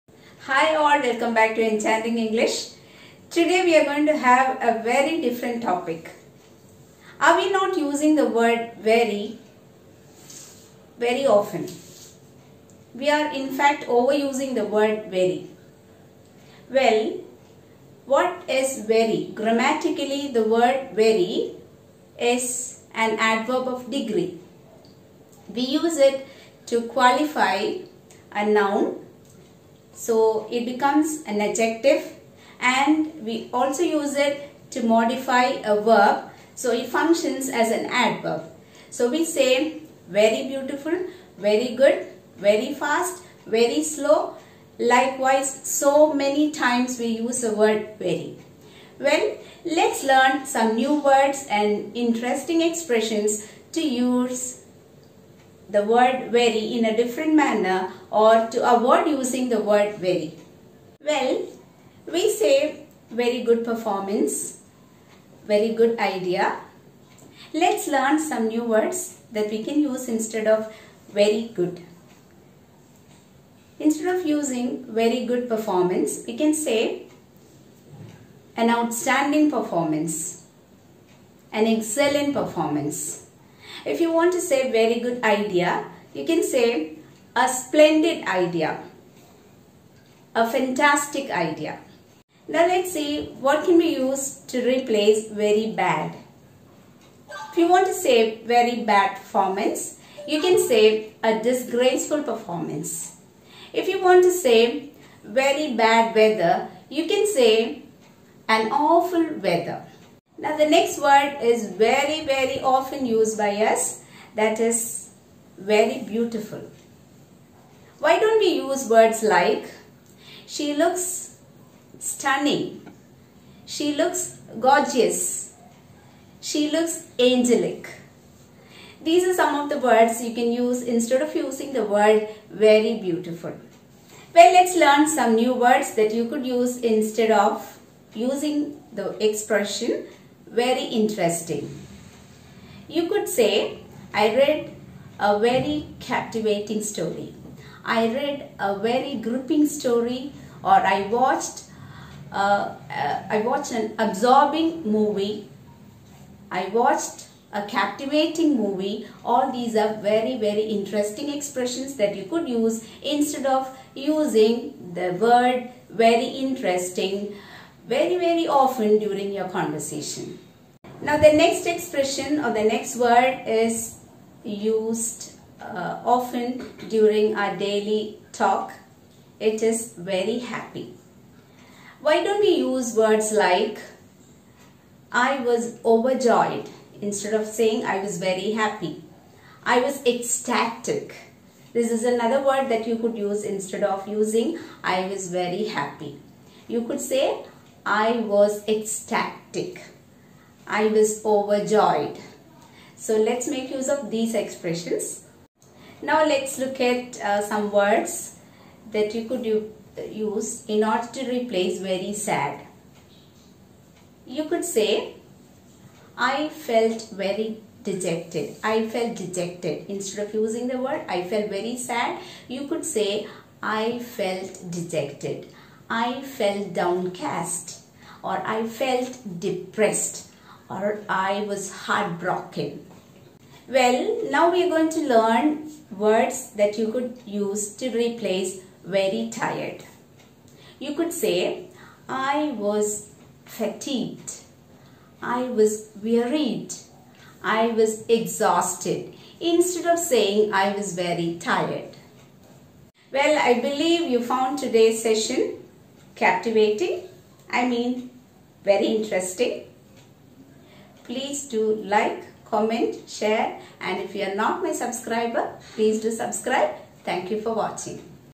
spokenenglish SMART VOCABULARY|USAGE OF ' VERY ' |ALTERNATIVE EXPRESSIONS
This video will enhance the vocabulary skill of of the viewers. Smart words and expressions to replace the word 'very' are given.